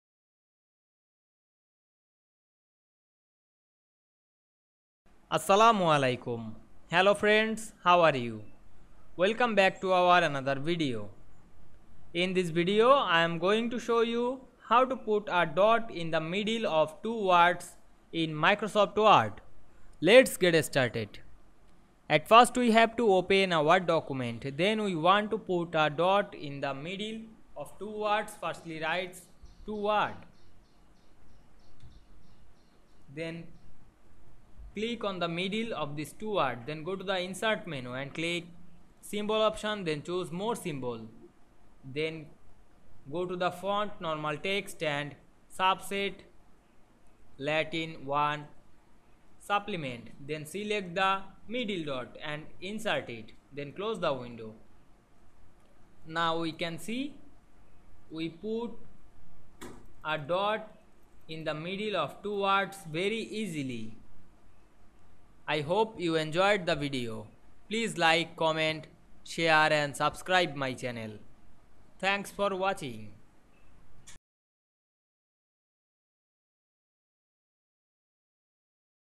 How to put a dot in the middle of two words in Word
Assalamu Walaikum,
In this video I will show you, How to put a dot in the middle of two words in Word. Let's get started.